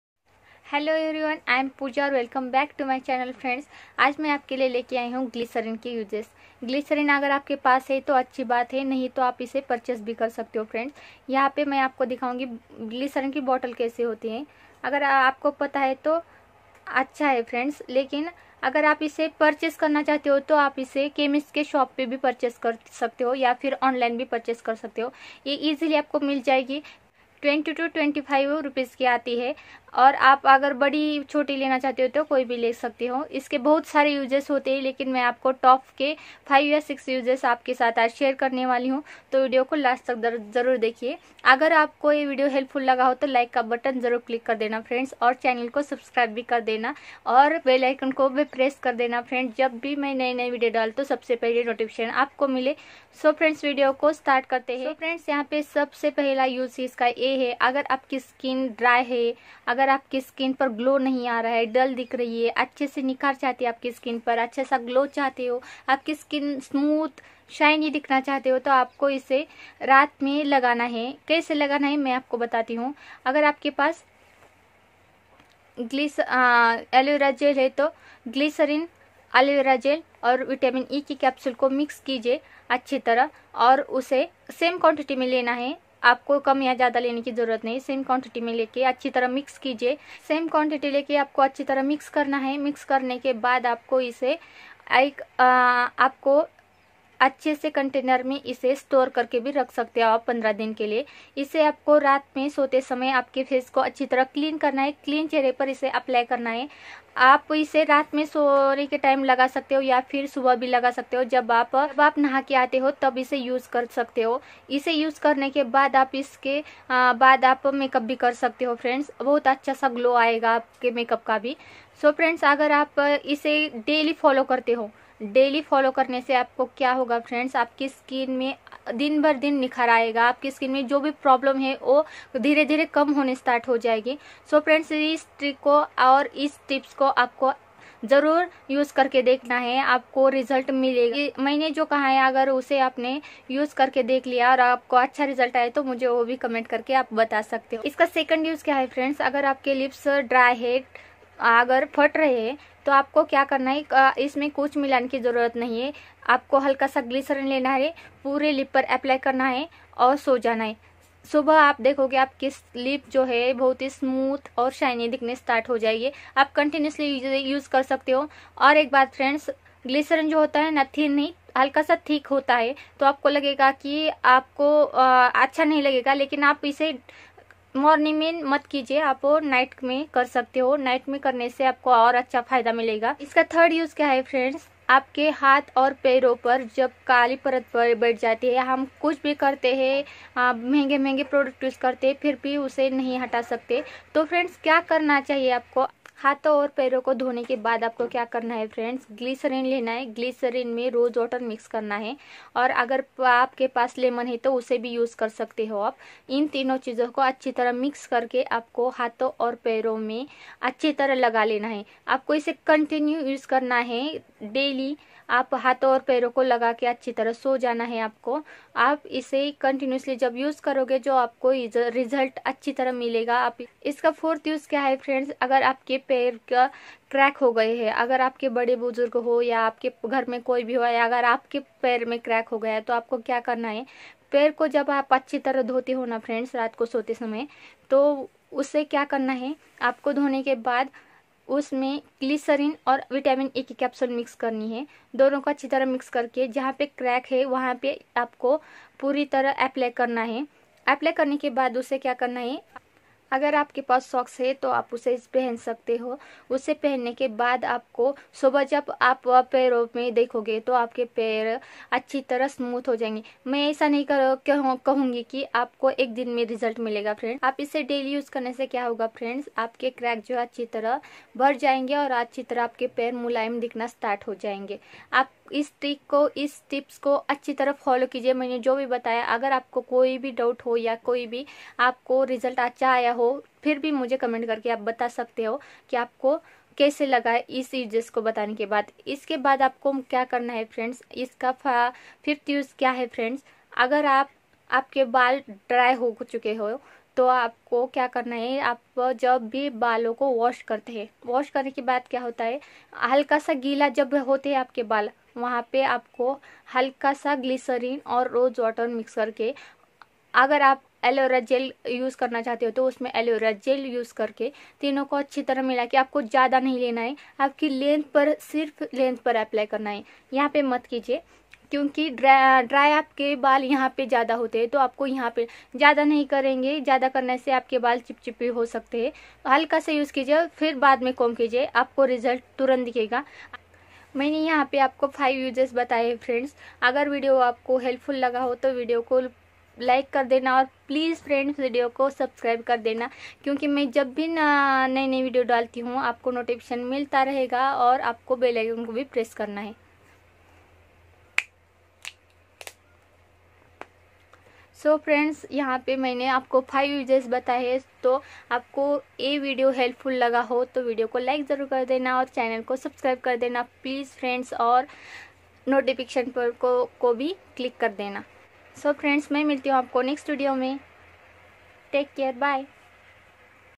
Top five uses of Glycerine/ग्लिसरीन के फायदे/How to apply glycerine on face/puja swami
Hellow everyone
l am a puja

Here is a new video where puja will show you top five uses of Glycerine and benifit of Glycerine.Its a simple home remedies which you can use on daily basis.A simple DIY hacks from puja swami.